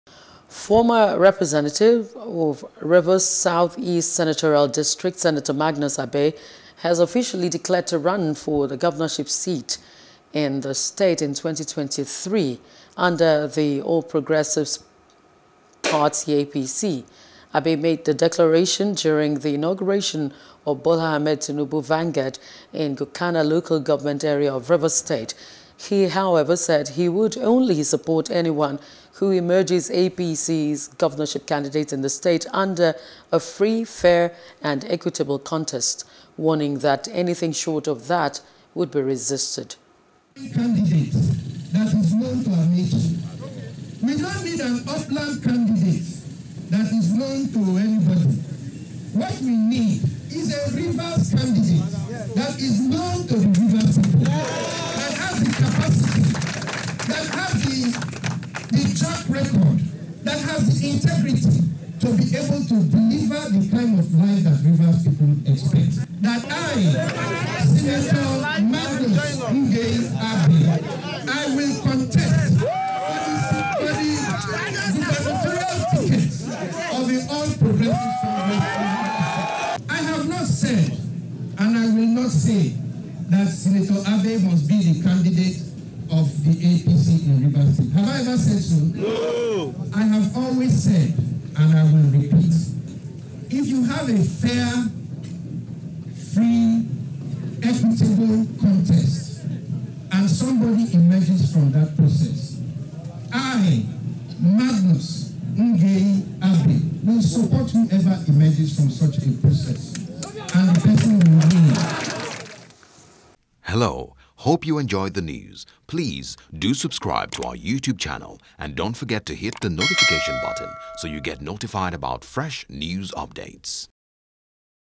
2023 Elections: Abe Declares For Rivers A.P.C. Governorship Race | NEWS
Former representative of Rivers Southeast Senatorial District, Senator Magnus Abe, has officially declared to run for the governorship seat in the 2023 elections in Rivers State under the All Progressives Party (APC).